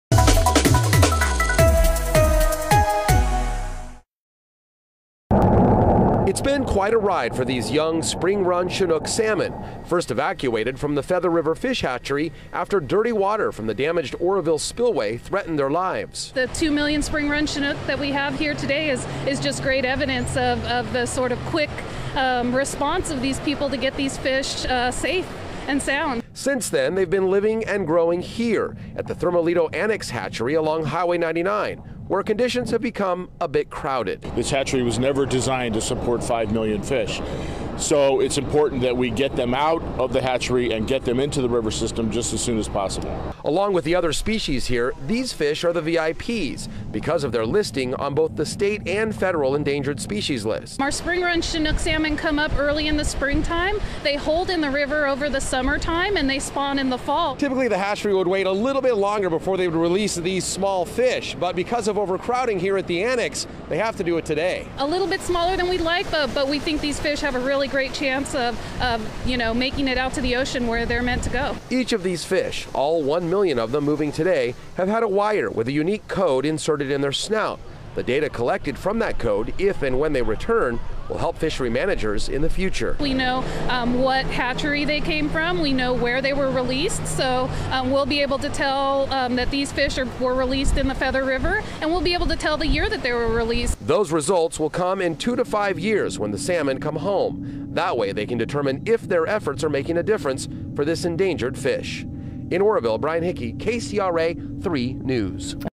Endangered Chinook salmon get new home in Feather River
One million endangered spring-run Chinook salmon that were moved during the Lake Oroville spillway emergency were set free Monday into their new habitat in the Feather River in Butte County.